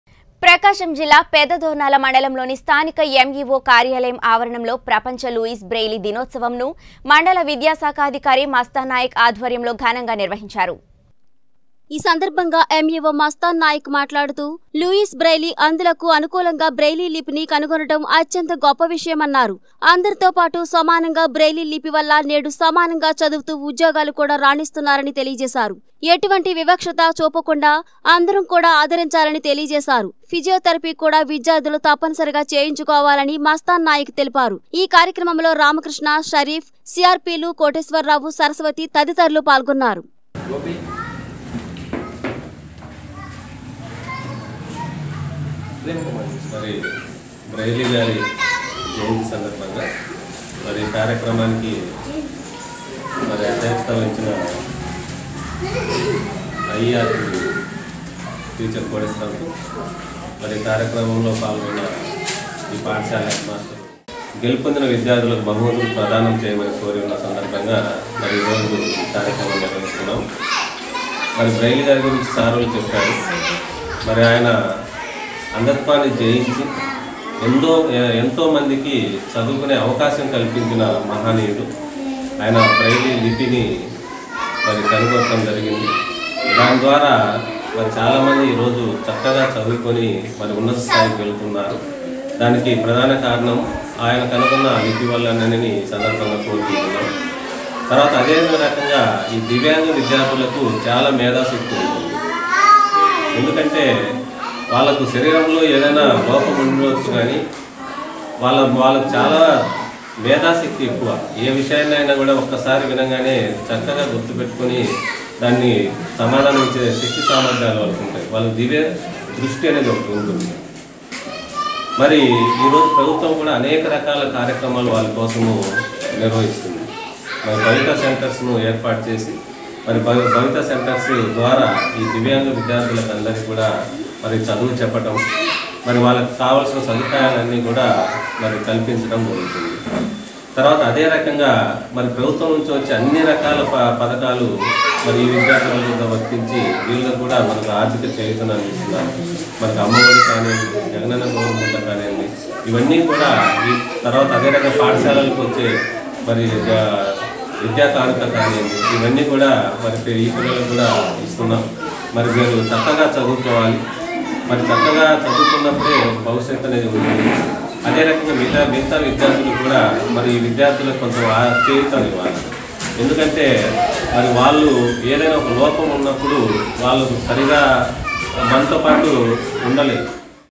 విద్యాశాఖ అధికారి మస్తాన్ నాయక్ ఆధ్వర్యంలో ఘనంగా ప్రపంచ లూయిస్ బ్రెయిలీ దినోత్సవం - ప్రకాశం జిల్లా
విద్యాశాఖ అధికారి మస్తాన్ నాయక్ ఆధ్వర్యంలో ఘనంగా ప్రపంచ లూయిస్ బ్రెయిలీ దినోత్సవం - ప్రకాశం జిల్లా | 19 Newstelugu